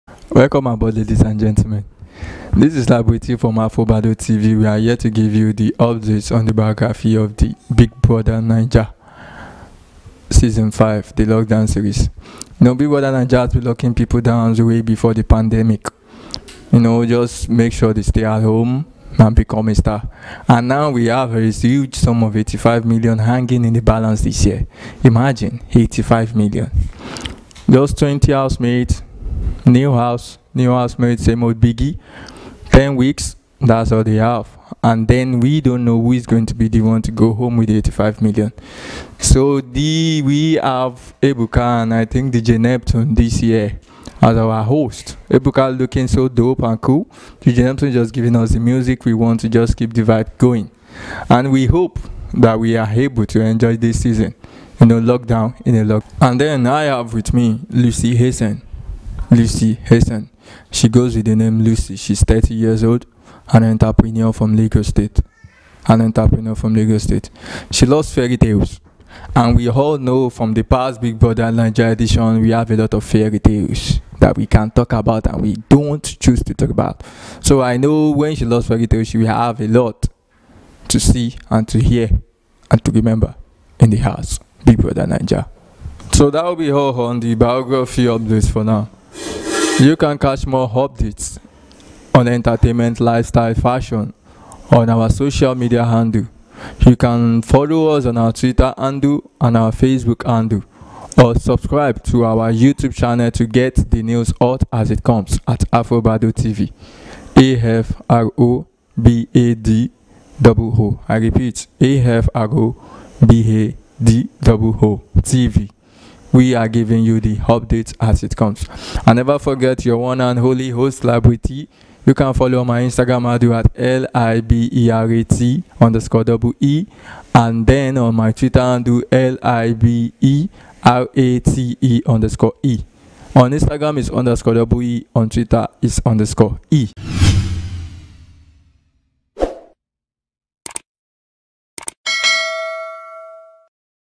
Lucy BBNaija Biography
We bring to you Lucy BBnaija Biography, this is all we know about the BBnaija housemate for now, we will be giving you more time goes on, so enjoy lucy bbnaija biography...